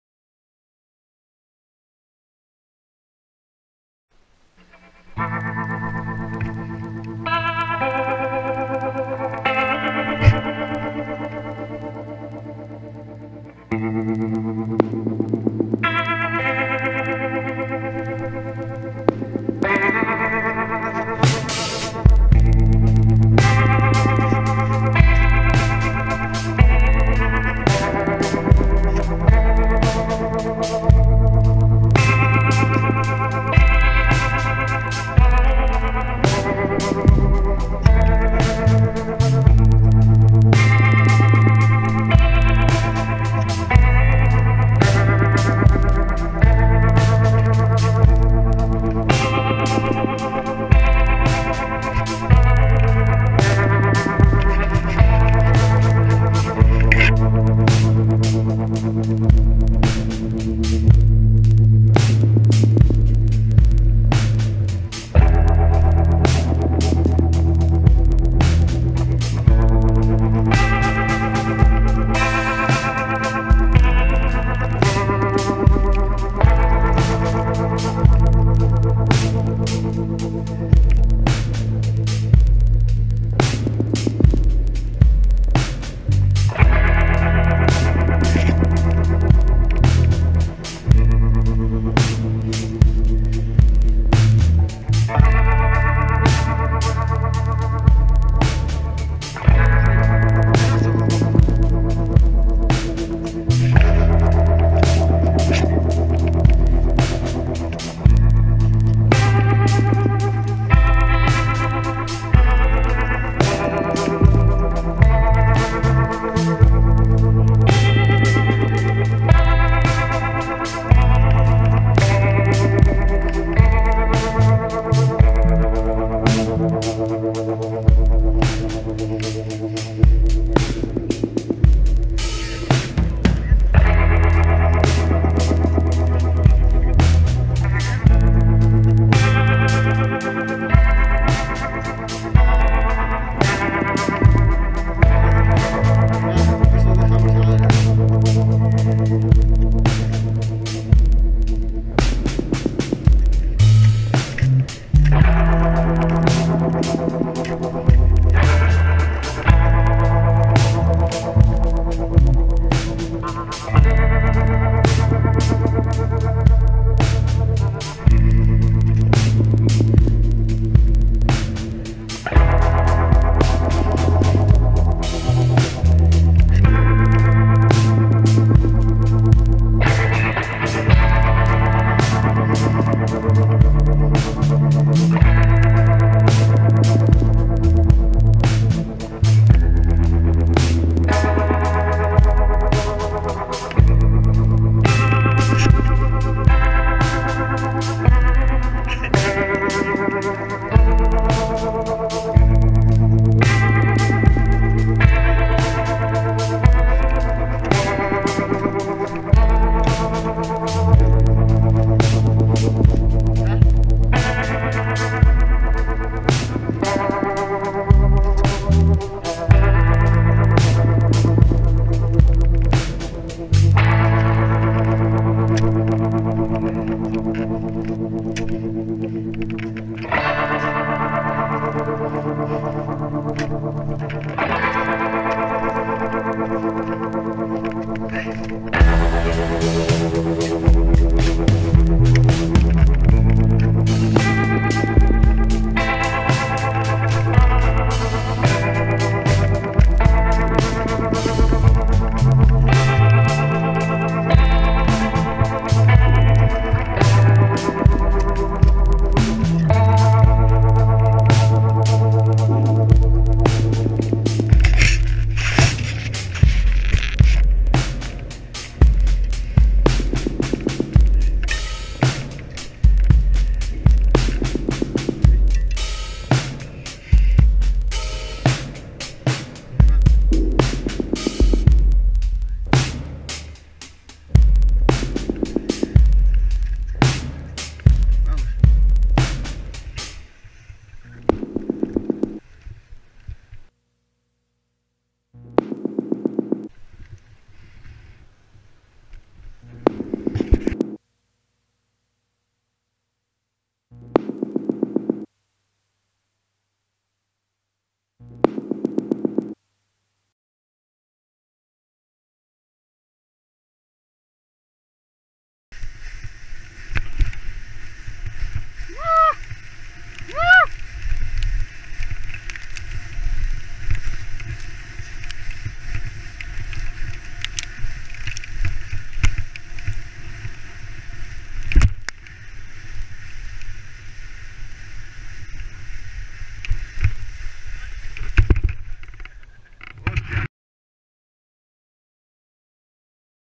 Clásica cara Norte Peña Ubiña 2019
Ascensión a Peña Ubiña (2417 m) por la Clásica Norte.